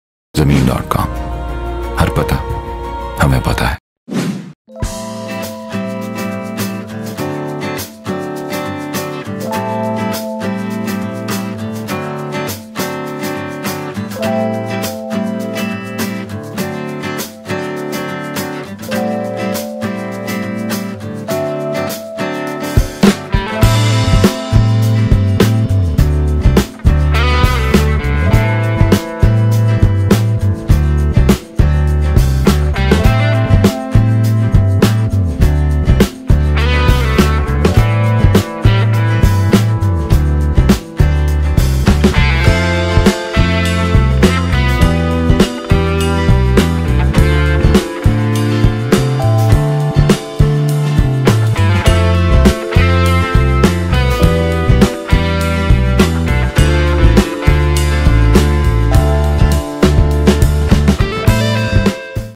5 KANAL FARM HOUSE FOR SALE IN DHOKE SYEDAN RAWALPINDI
5 Kanal Farm House Plot Is Available For Sale

100% level plots, Fertile & lush land.
All facilities will be available.
Maverick Farms is a farm housing project located near CPEC Interchange Dhoke Syedan.